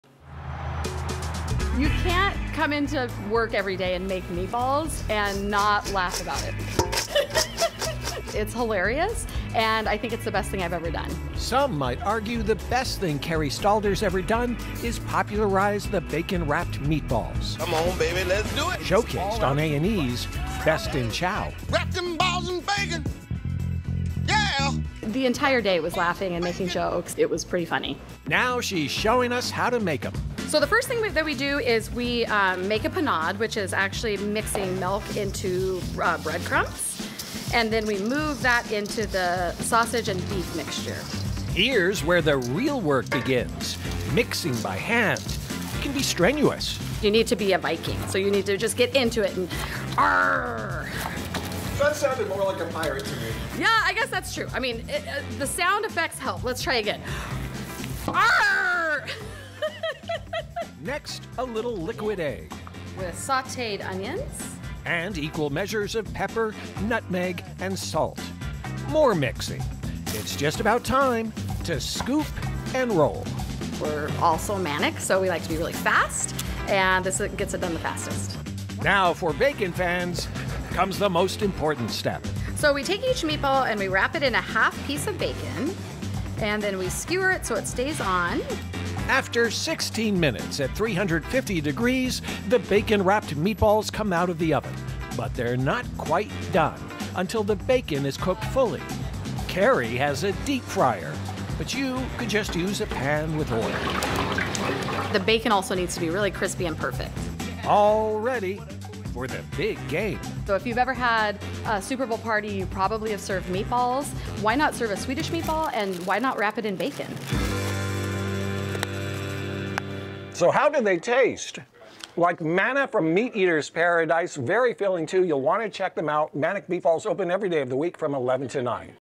Manic Meatballs is home to the ultimate Super Bowl party snack
The Seahawks won't be playing but you can win your Super Bowl party with this famous bacon meatballs recipe.